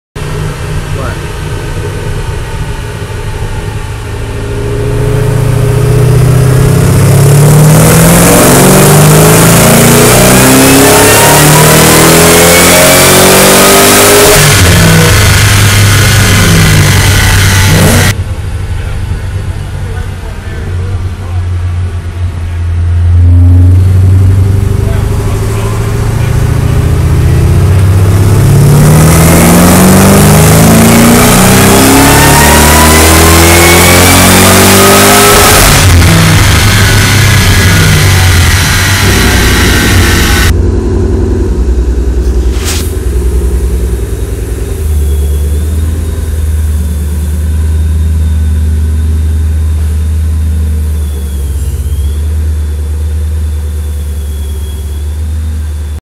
SUPER SNAKE IN CAR DYNO 1080P
PCM dyno day 2012, Shelby Super Snake 725 model, made 630rwhp @ 6125rpm and wanted to keep going!